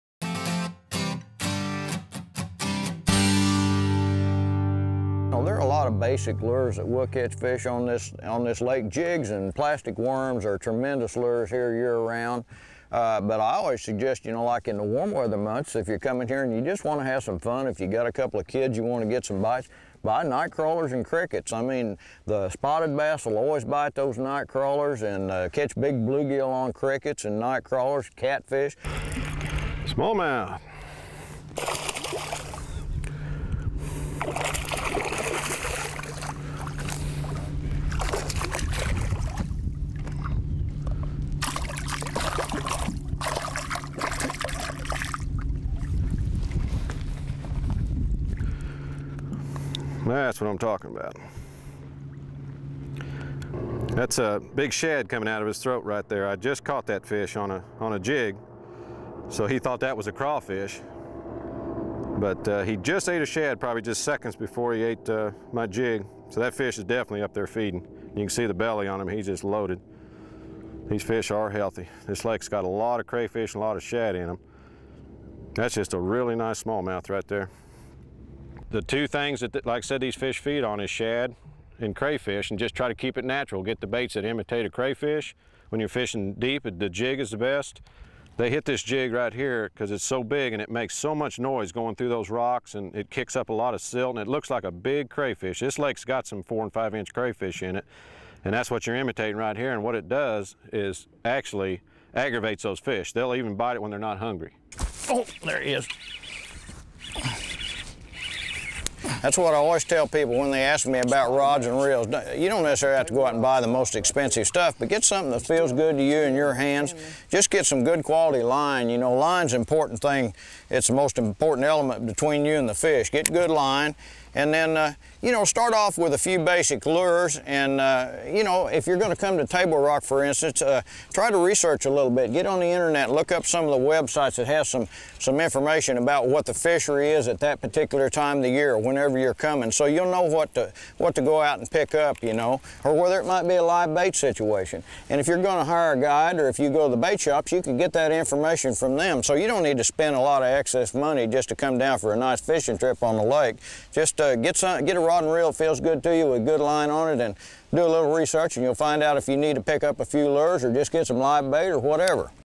Fishing with the Pros - Part 6 "Baits, Lures & Equipment"
Get tips on how to fish Table Rock and Bull Shoals Lakes in Missouri from fishing pros Rick Clunn, Stacey King, Tim Sainato, Brian Snowden and Mike Webb.